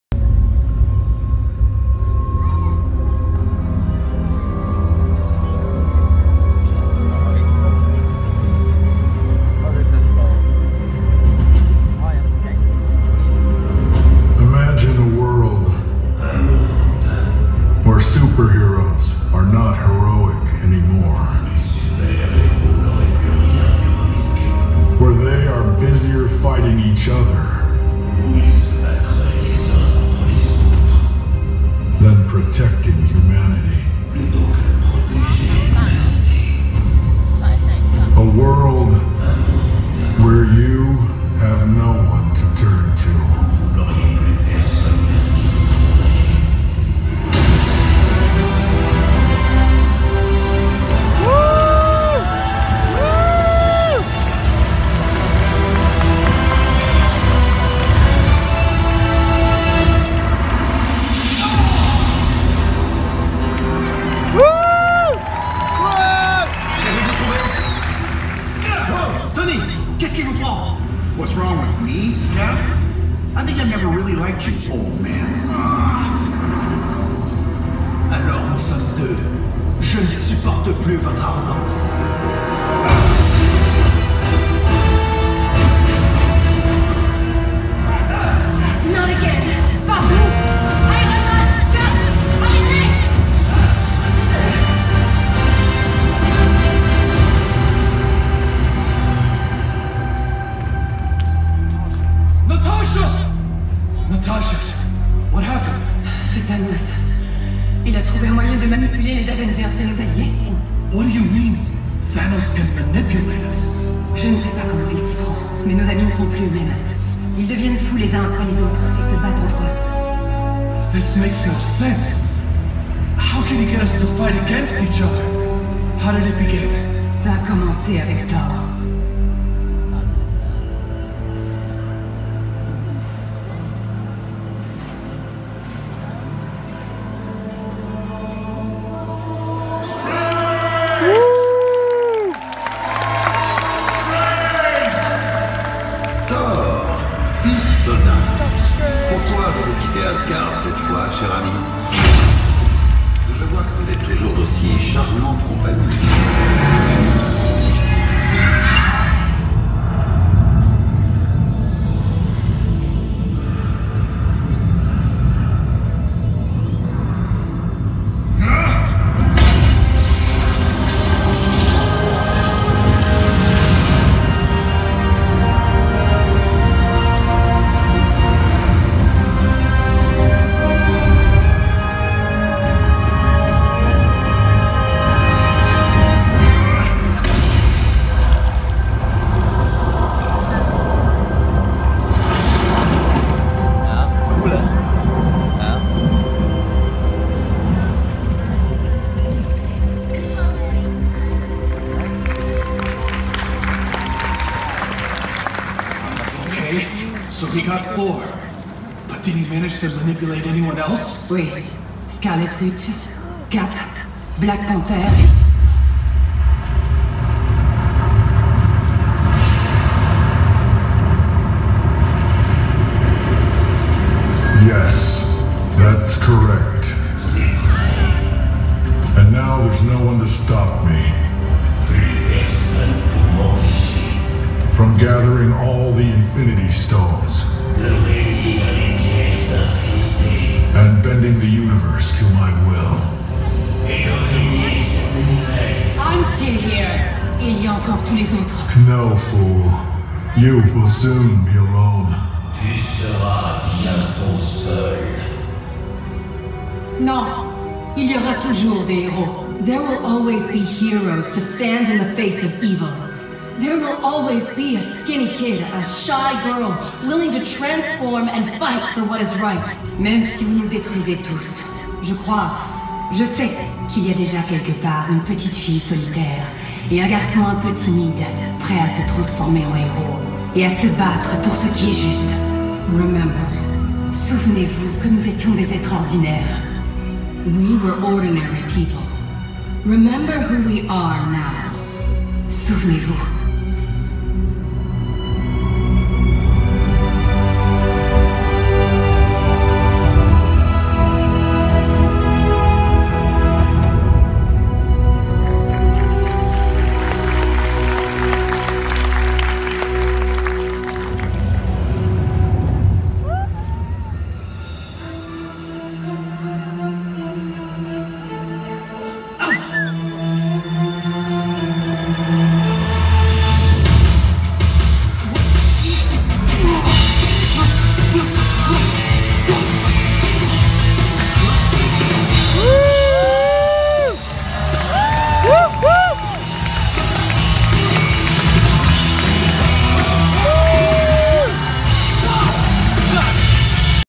Marvel Show part 1, Disneyland Paris, 2018
First row view at the Marvel Summer of Superhero show this year at Disneyland Paris. Borrowing from the movies, this show creates its own story of the avengers and there fight against Thanos
All rights go towards Disney/ Marvel